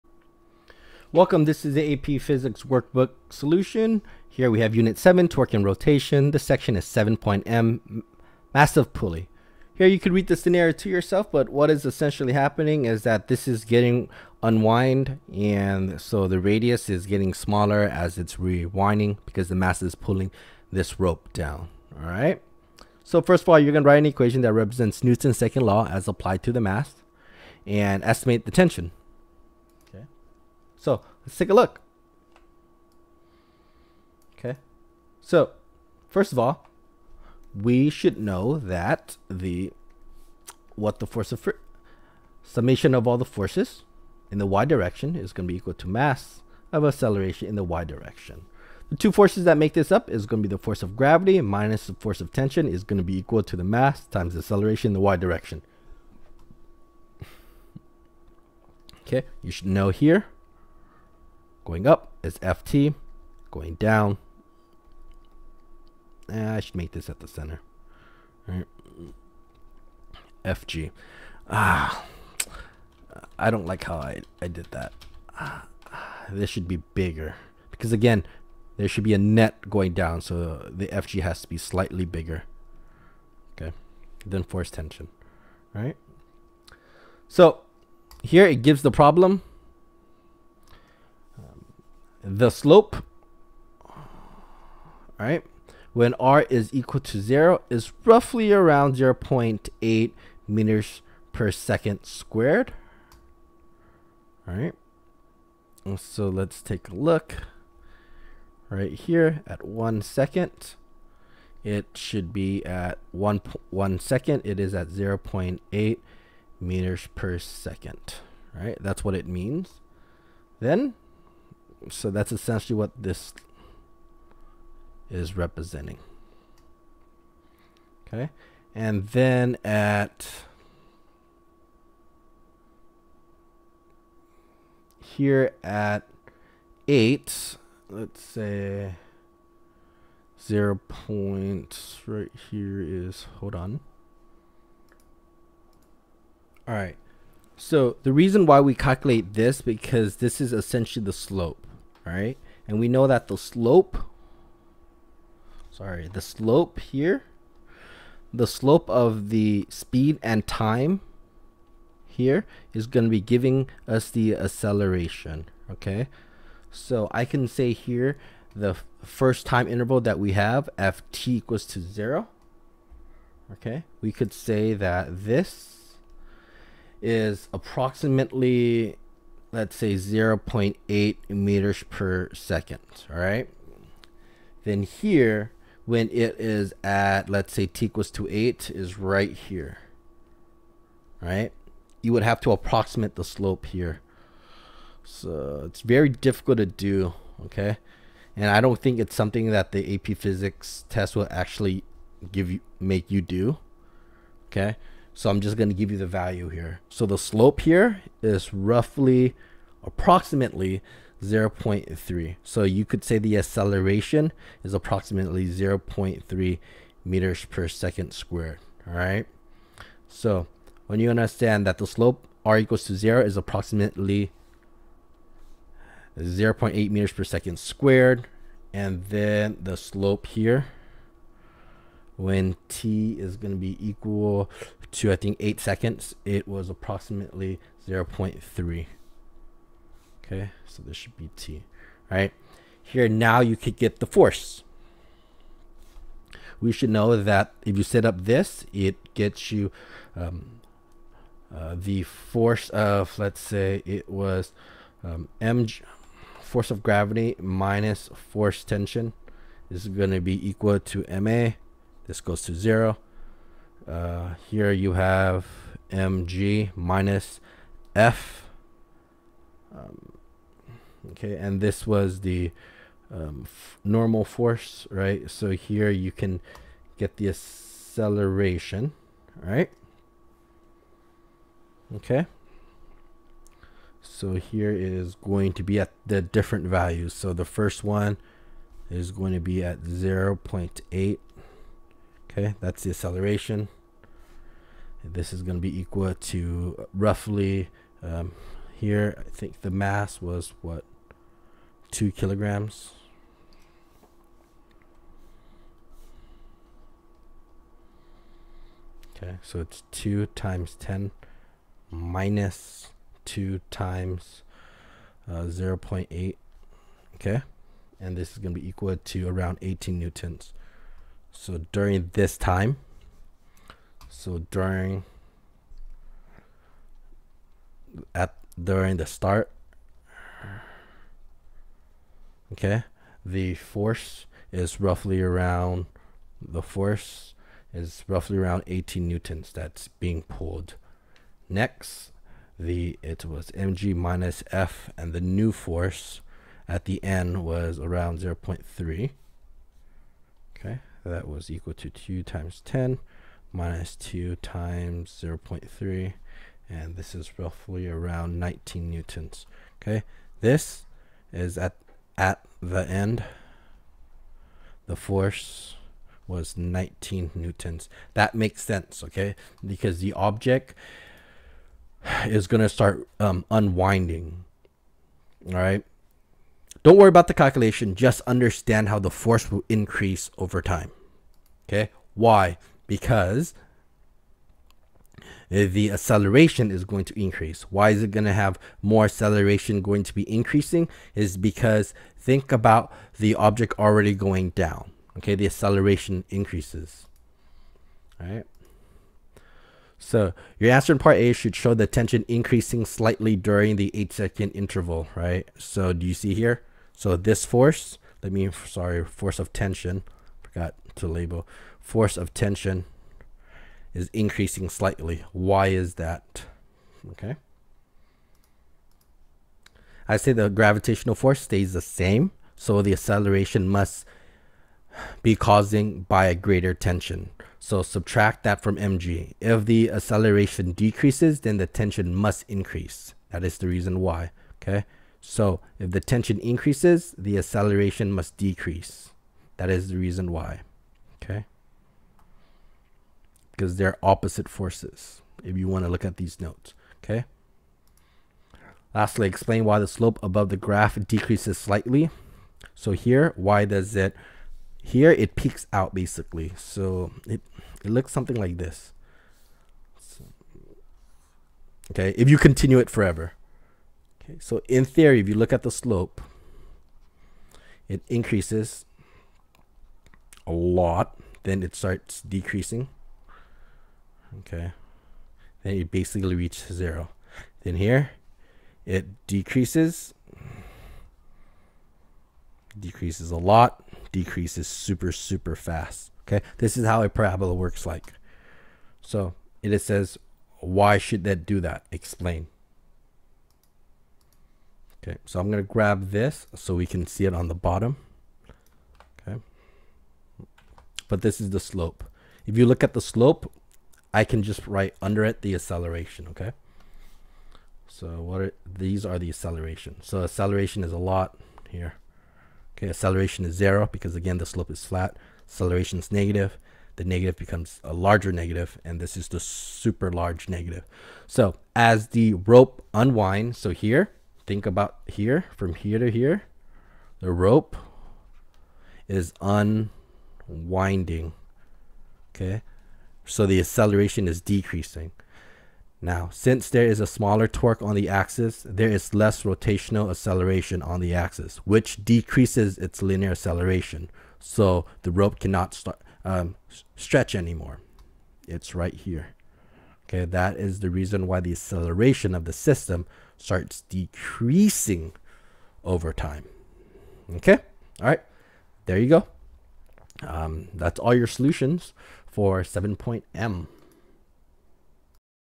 AP Physics Workbook 7.M Massive Pulley
This is the video that cover the section 7.M in the AP Physics 1 Workbook.
Topic over:
1. Torque changes over time

This is a packet given to student by the CollegeBoard and for student to do. Student are to give the packet an honesty  attempt and effort. This video is created to supplement the learning and to provide assistance to student in the learning process and is not suppose to be use as a cheating tool.